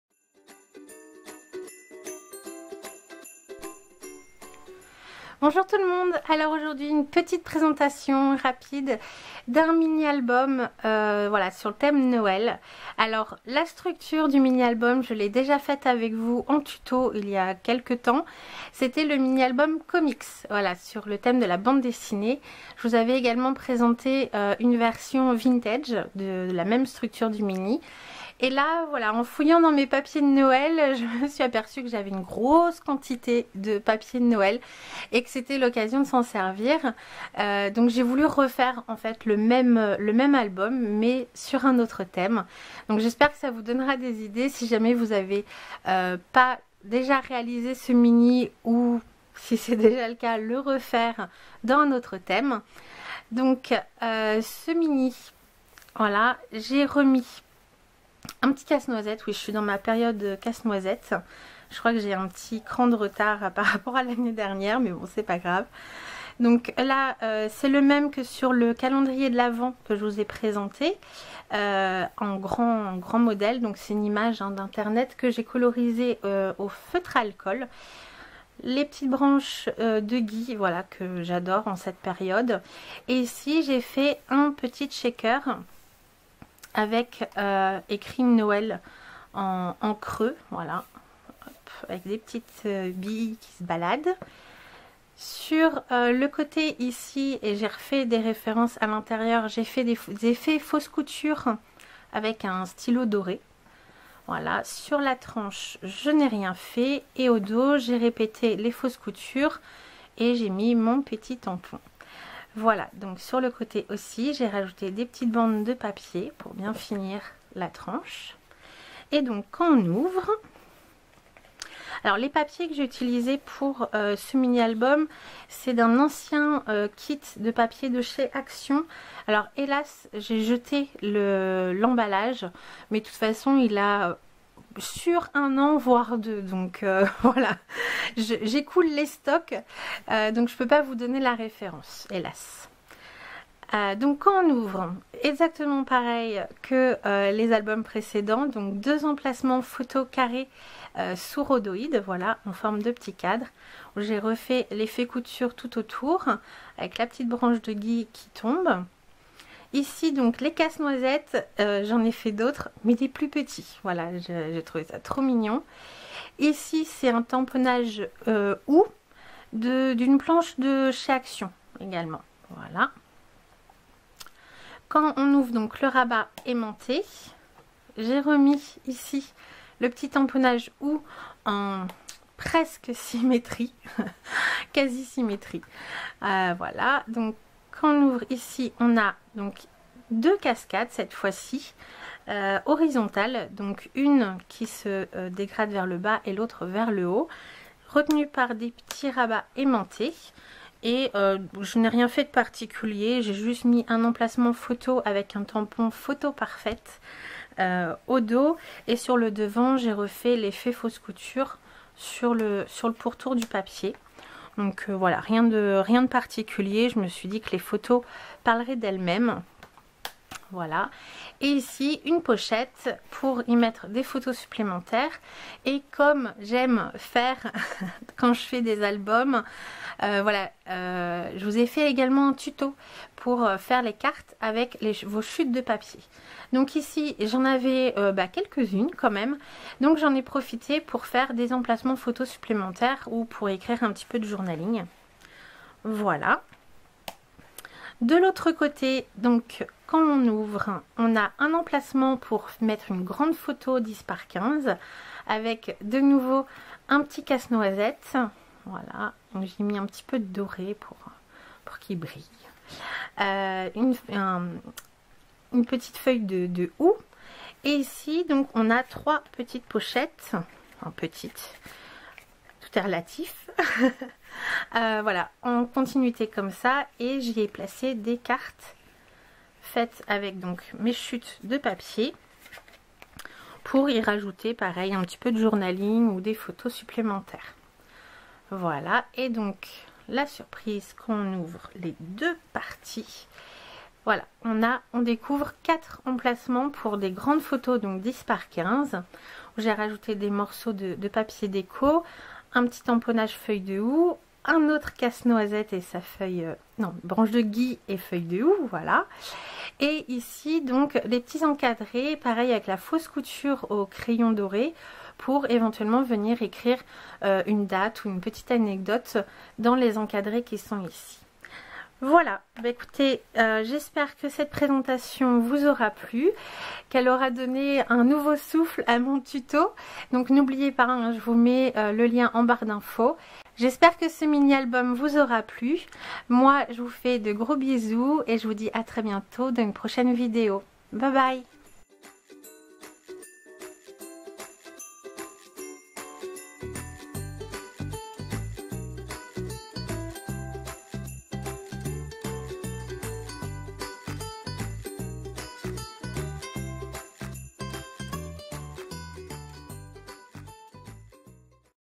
Mini album portfolio #3 Noël : présentation
.POUR PLUS D'INFORMATIONS, DEROULEZ ICI.

En pleine préparation de Noël, j'ai voulu reprendre un de mes tutos (et oui je visionne moi même mes vidéos!!!). J'ai revisité le mini album portfolio que je vous avez présenté en version comics et vintage (voir liens ci-dessous) mais cette fois en version Noël!!
J'espère que cela vous donnera des idées pour conserver vos précieux souvenirs ou des idées cadeaux 😉😊
Je vous souhaite bon visionnage et surtout bon scrap !!!

N'hésitez pas en cas de besoin ou simplement pour combler ma curiosité à m'envoyer des photos de vos réalisations!! à cette adresse mail

Et très nouvellement sur Vinted vous y trouverez un peu de tout mais aussi des minis albums!